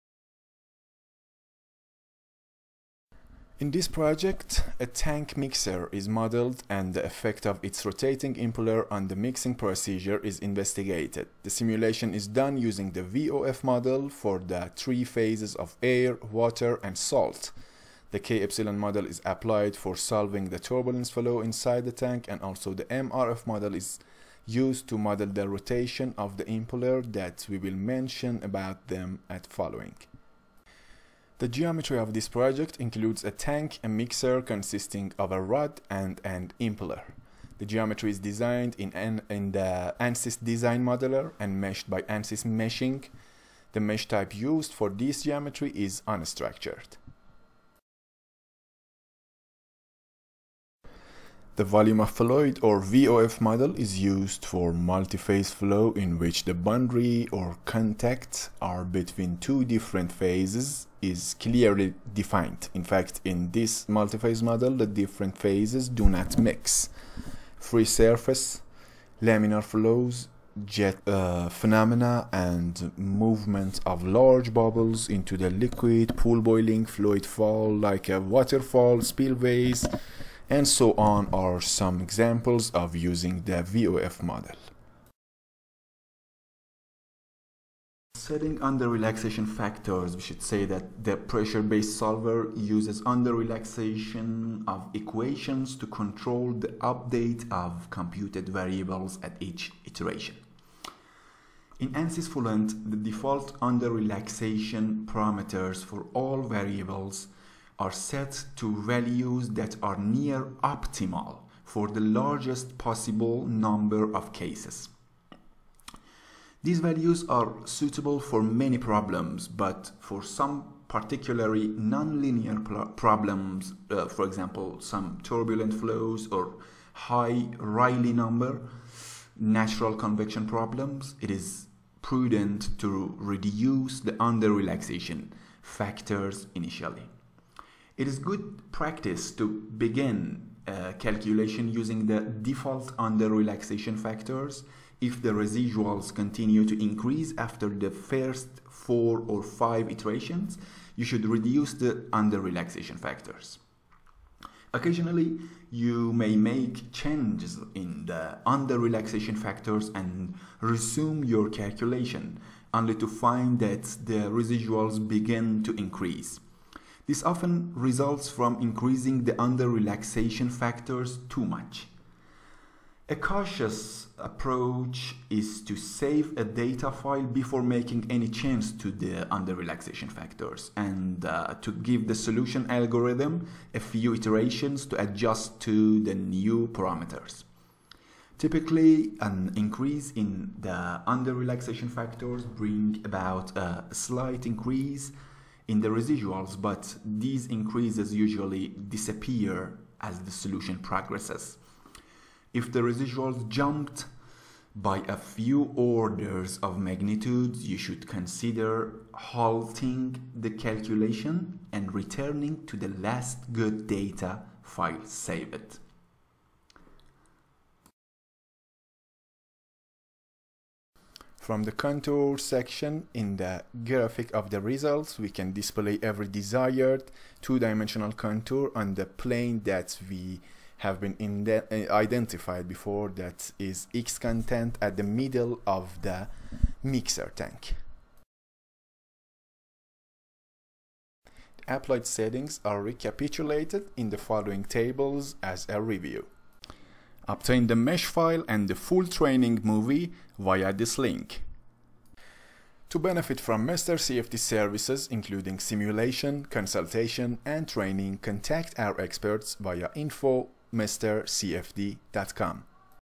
Mixer Tank CFD Simulation
In this project, a tank mixer is modeled and the effect of its rotating impeller on the mixing procedure is investigated. The simulation is done using the VOF model for the three phases of air, water, and salt. The k-epsilon model is applied for solving the turbulent flow inside the tank. MRF model is also used to model the rotation of the impeller.